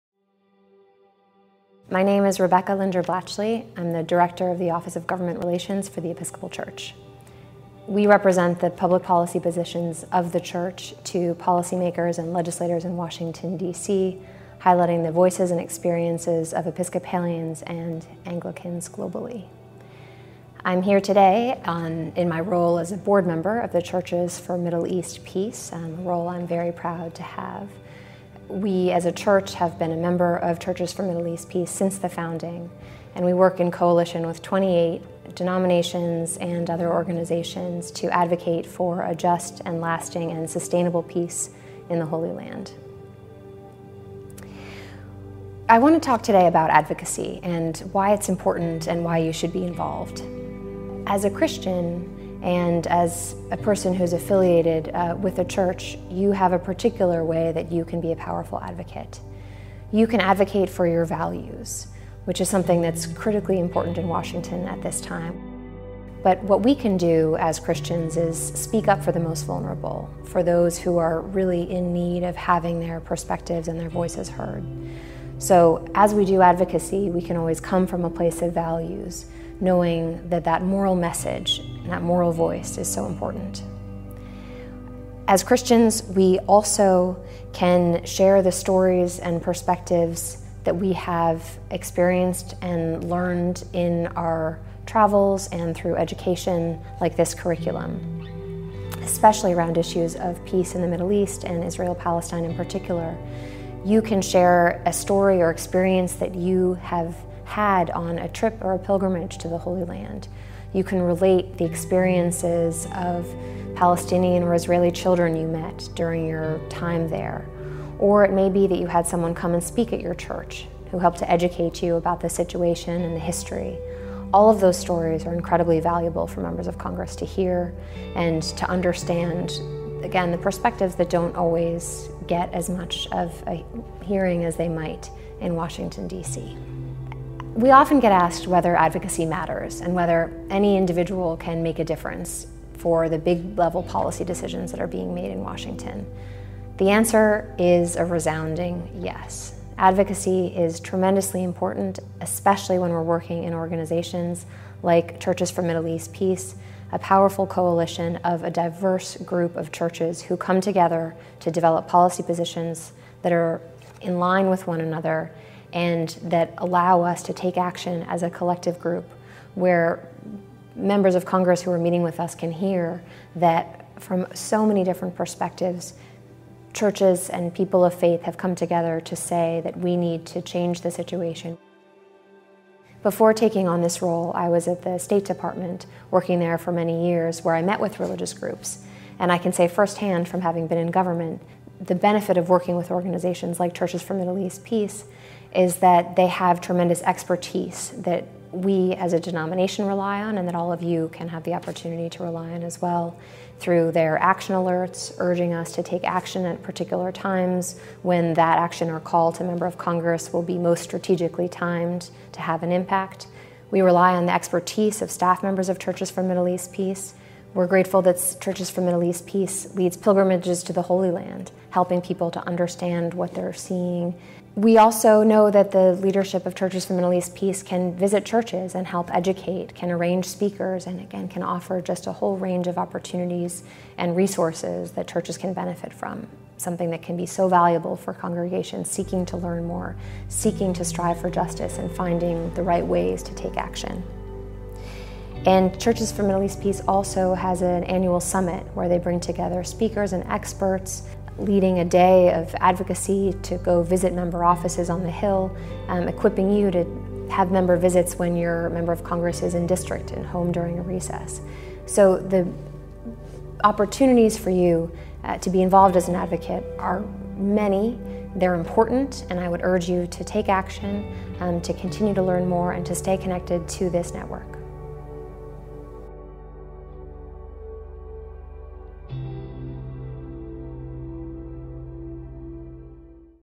The Search for Peace & Justice in the Holy Land: Faith in Action with Rebecca Linder Blachly
Rebecca Linder Blachly is the Director of the Episcopal Church's Office of Government Affairs and is also a member of Churches for Middle East Peace's (CMEP) board of directors. Here she speaks of the importance of Christians advocating their faith-based values to their representatives in Washington, D.C.

“What we can do as Christians is speak up for the most vulnerable, for those who are really in need of having their voices heard.”

Learn more about Christian advocacy with CMEP's Curriculum. for more.